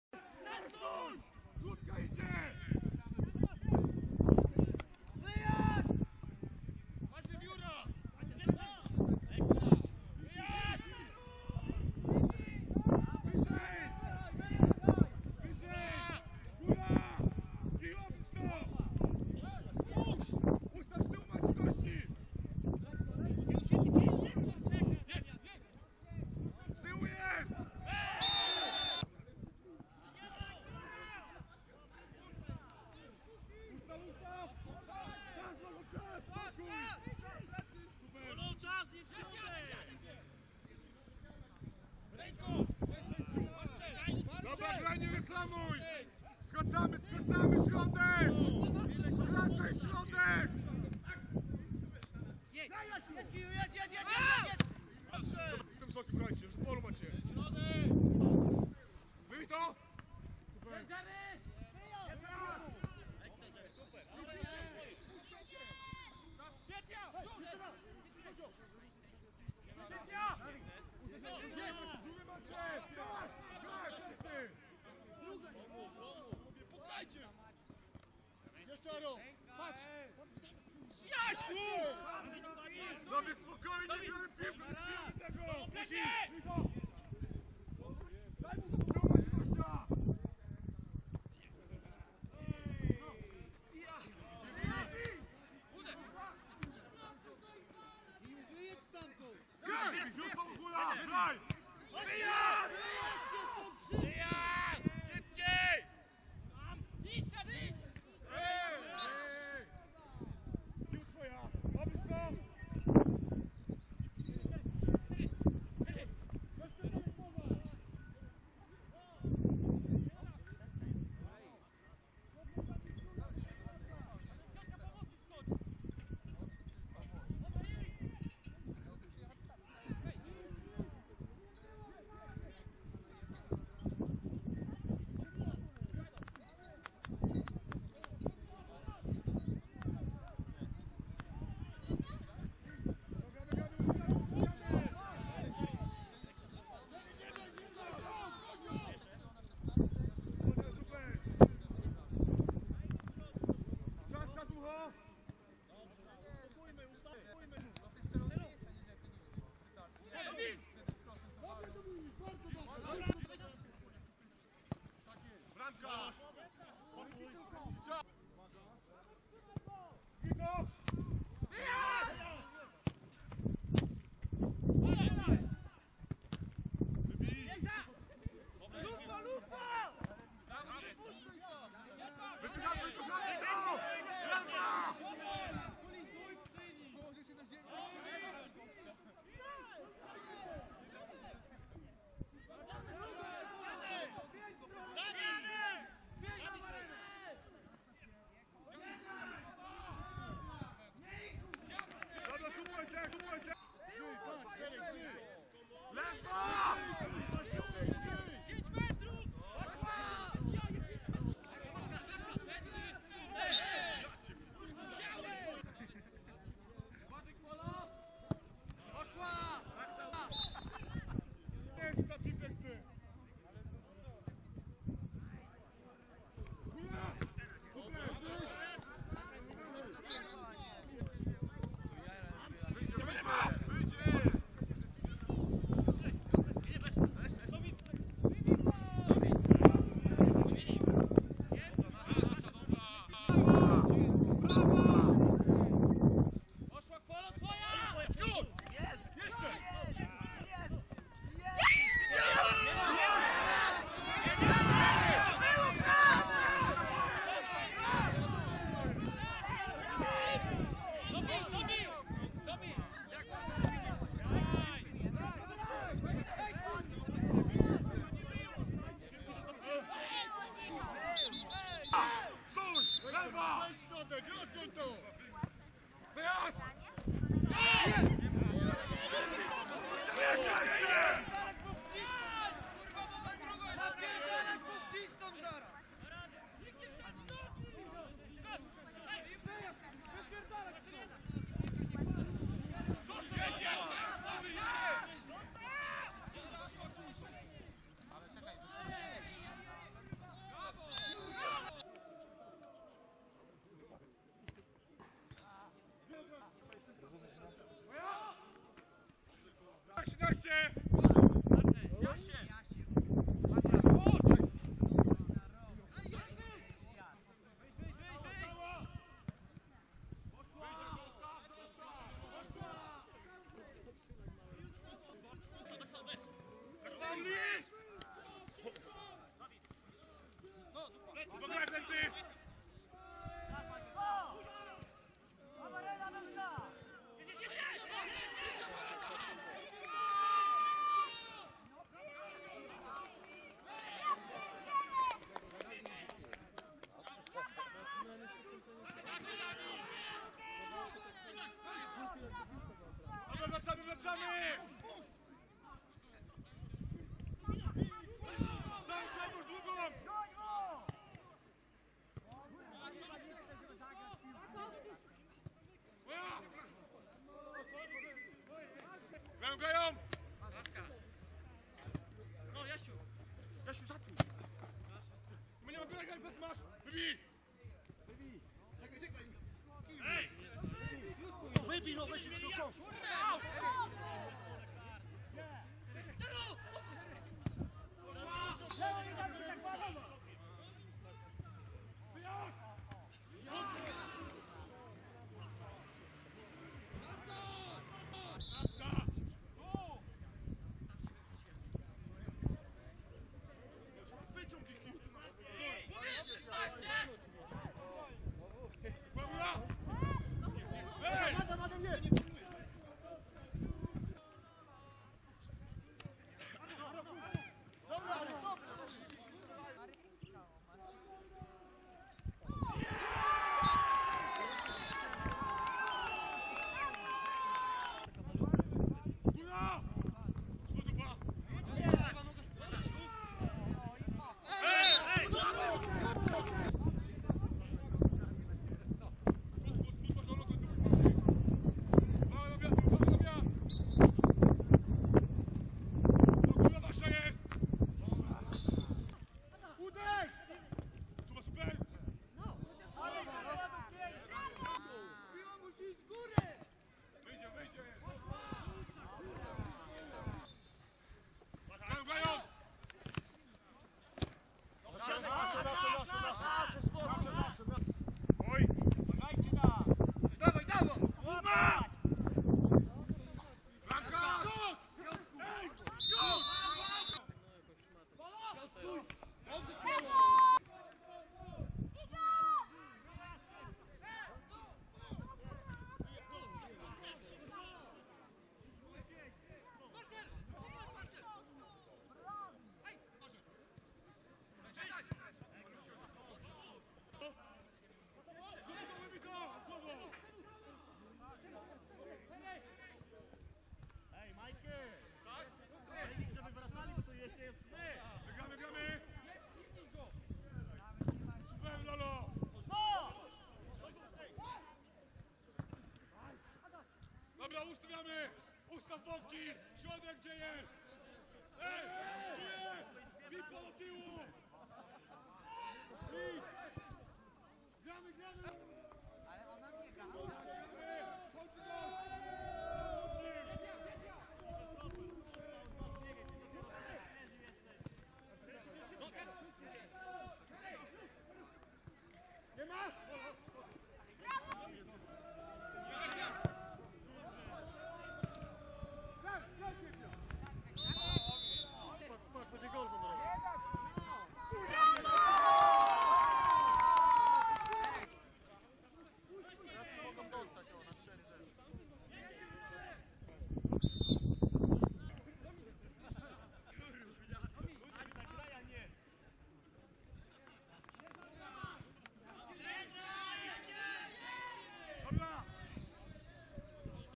Mecz ligowy B-klasy: Jawornik Czarna - Pogórze Srogów Górny (0:3)
Mecz ligowy Jawornik Czarna kontra Pogórze Srogów Górny.
Wynik: 0:3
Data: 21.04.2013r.
Miejsce: Czarna Górna